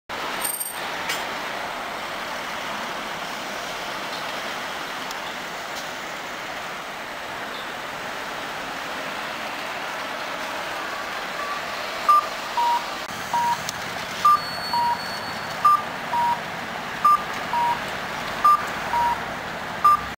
2021.01.12 いすゞ エルガ 2DG-LV290N2 近鉄バス 5956 (鳥飼営業所所属) 大阪200か47-36 ｢73 花園･東和苑｣｢回送｣ 阪急茨木バス停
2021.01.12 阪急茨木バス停
いすゞ エルガ 2DG-LV290N2 ノンステWB5.3m
大阪200か47-36 (近鉄バス 鳥飼営業所所属 5956)
｢73 花園･東和苑｣
｢回送｣